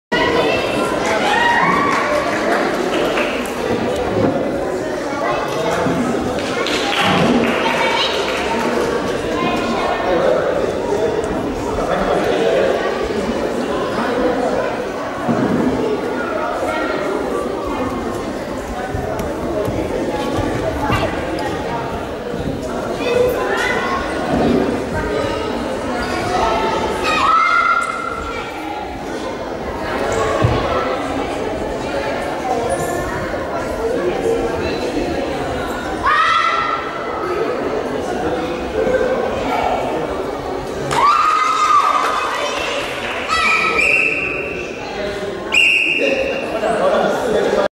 wrka kyoso cup 2017 11 04 rg2 division f11 9 - 11yrs female kata beginner 2b unknown heian shodan
2017 WRKA Kyōsō Cup Karate Championships

04 November 2017

Ring2: Division F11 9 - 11yrs Female Beginner Kata

Some Kata may not be accurately identified by there correct name for the style of the competitor
or I have been unable to hear what was called and find a matching kata